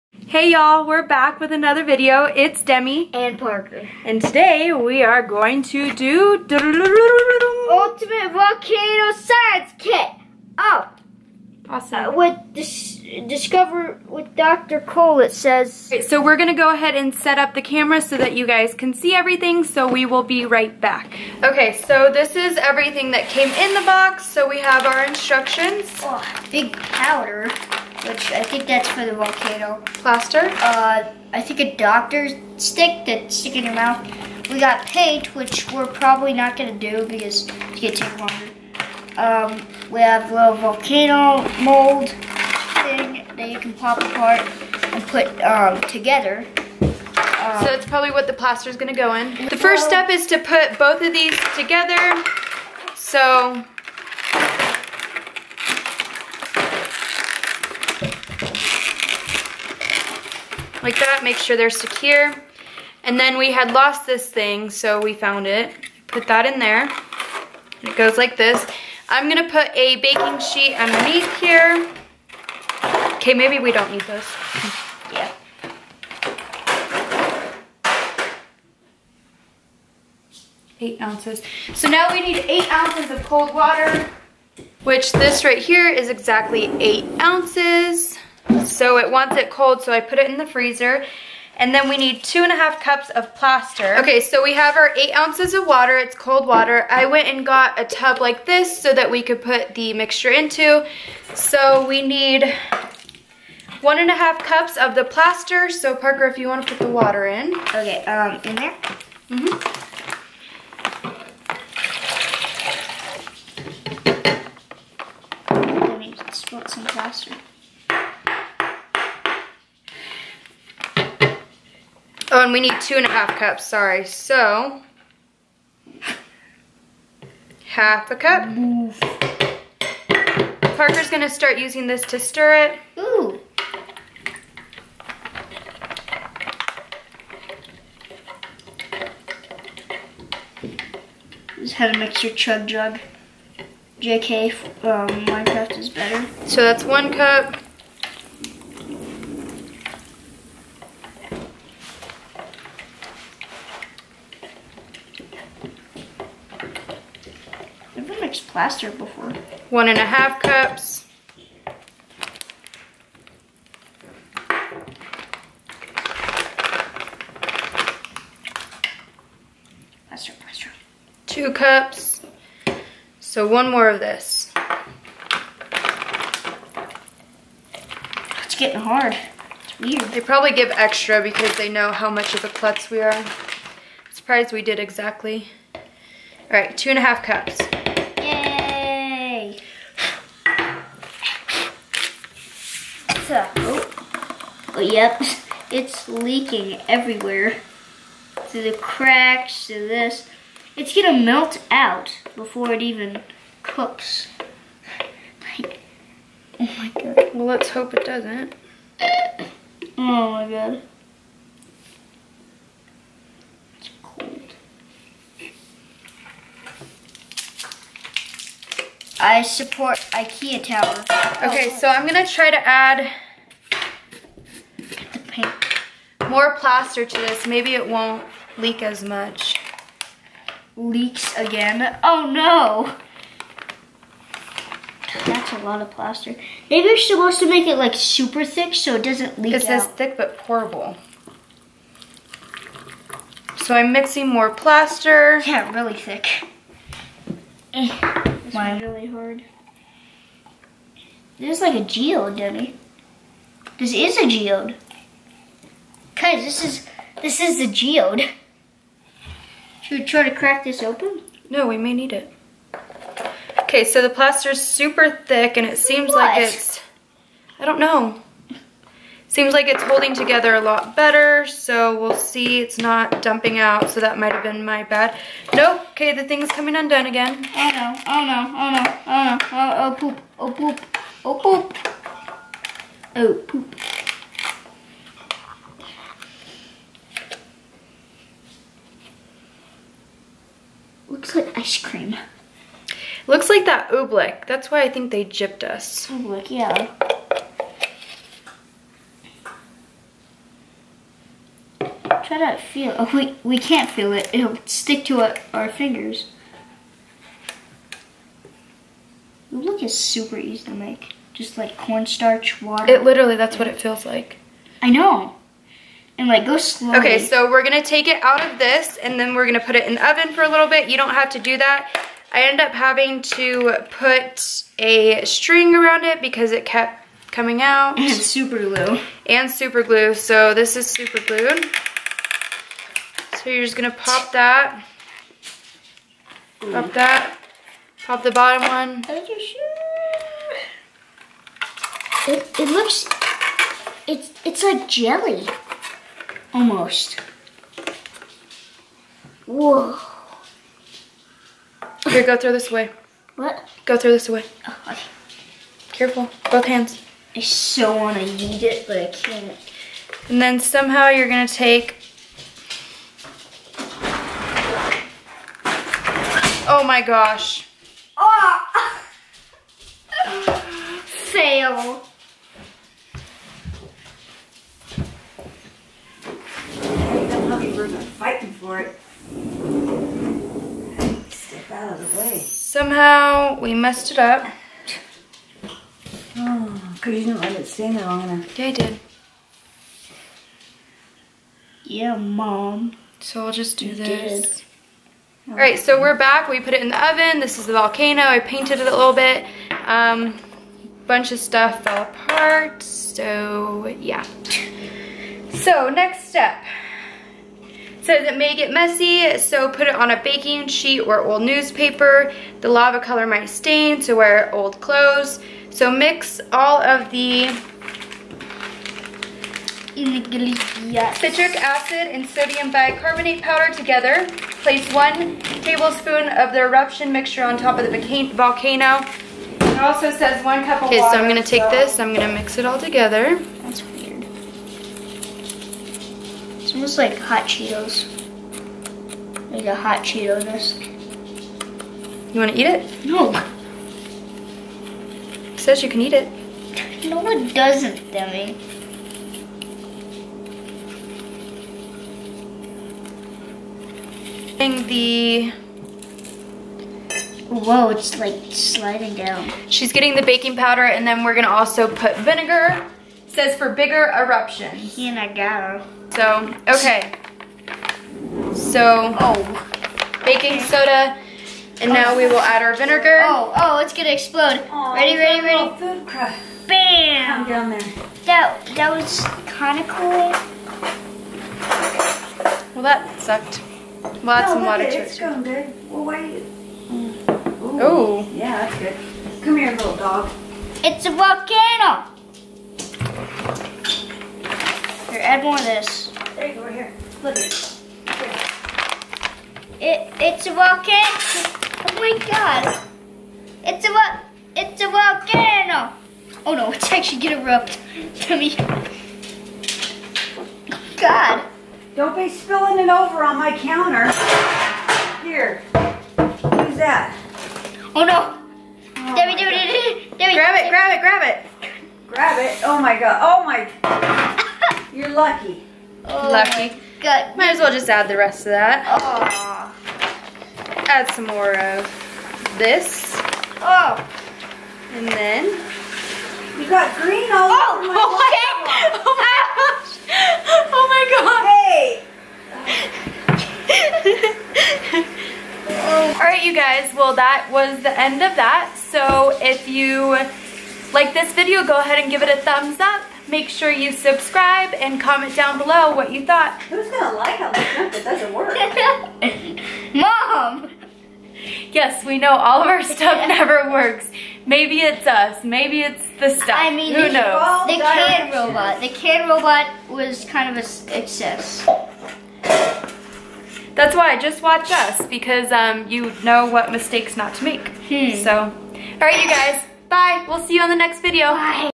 Volcano Fail.. Once Again.. Nothing New
Hey ya'll its Demi and Parker with another video.
Watch us fail at making this exploding volcano!

The kit we used is call "Ultimate Volcano Science Kit" which you can purchase on amazon.